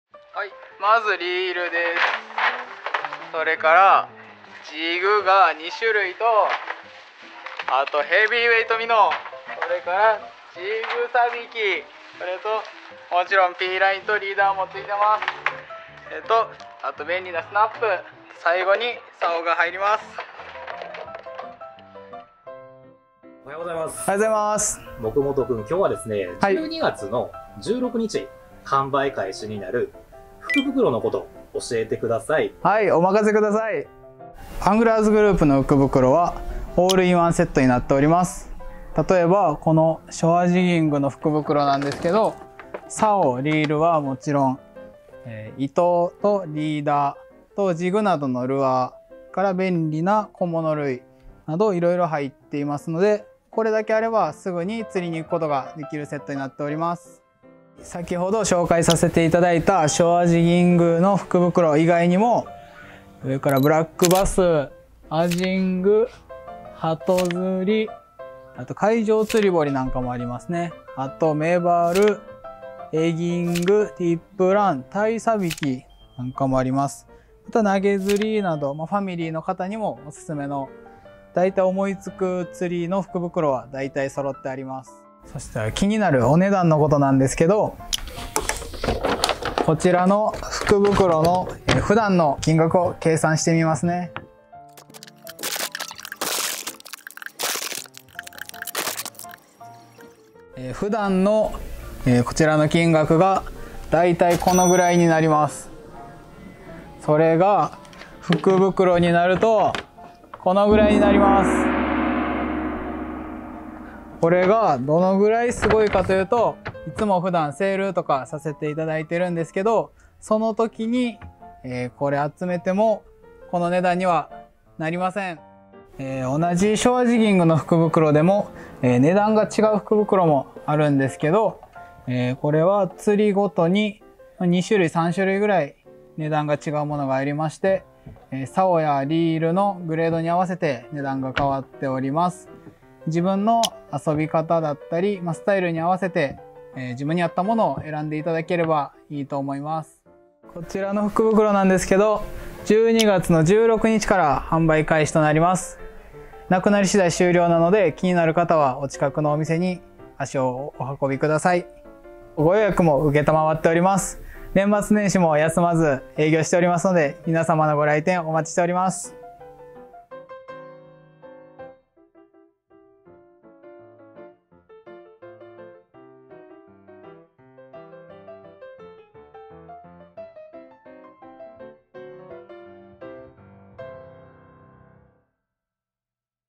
【2023年釣具店の福袋】大好評オールインワン福袋のおススメポイント!
今年もやります“アングラーズグループのオールインワン福袋”
スタッフがおすすめポイントを説明してくれました!
プレゼントにも最適な福袋なのですが
一番の衝撃はスタッフも驚く値引き幅で・・・